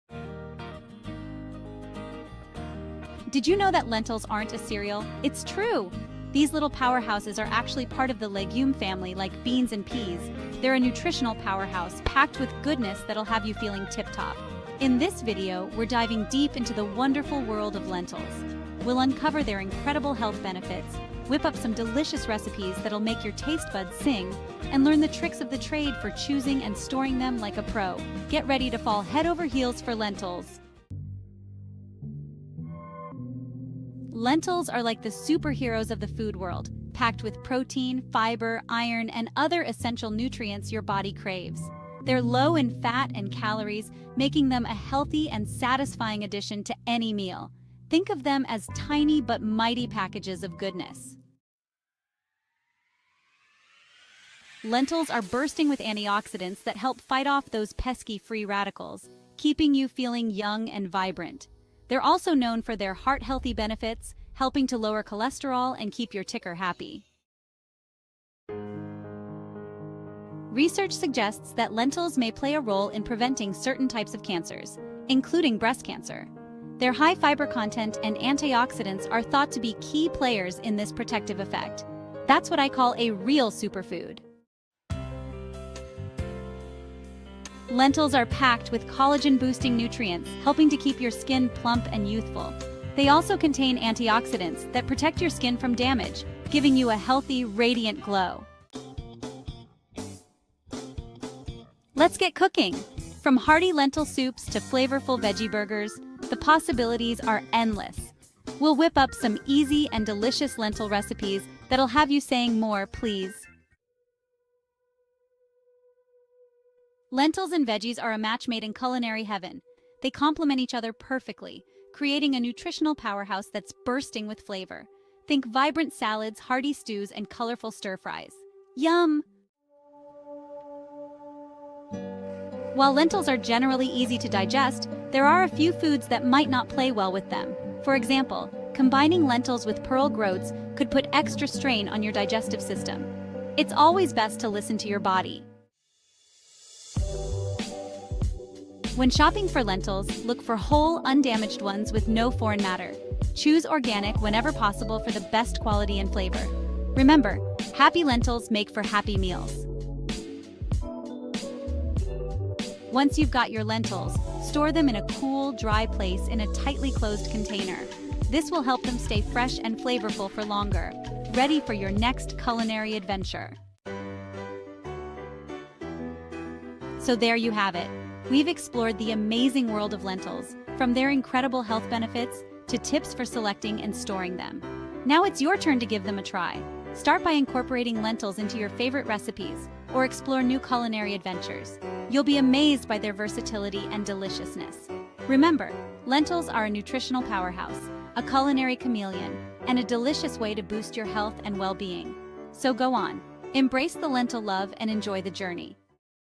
Lentils are a legume crop they have a number of health benefits and are used in a variety of recipes
Lentils are not a cereal, but belong to leguminous crops. Nevertheless, it has a number of beneficial properties and can be used in a variety of recipes:

### Benefits of lentils

- Lentils are rich in protein, fiber, vitamins and minerals.
- It has antioxidant, antibacterial, cardio-protective, antifungal and antiviral properties.
- Lentils can help in the prevention of breast cancer.
- It slows down the aging process and improves skin health.

### Recipes with lentils

- Lentils can be used to make delicious pancakes, puree soup, and desserts.
- Lentils are good to combine with fresh vegetables, which help the body to absorb its nutrients.
- It is not recommended to combine lentils with pearl groats, as this can put a strain on the body.

### Selecting and storing lentils

- When choosing lentils, pay attention to their appearance - they should be whole, without damage or foreign matter.
- You can also evaluate the smell of lentils - good products usually have a nutty flavor.
- Lentils are best stored in a cool, dry place in a tightly closed container.

Thus, lentils, although not a cereal, are a valuable product rich in nutrients and useful properties. It can be used in a variety of delicious and healthy dishes.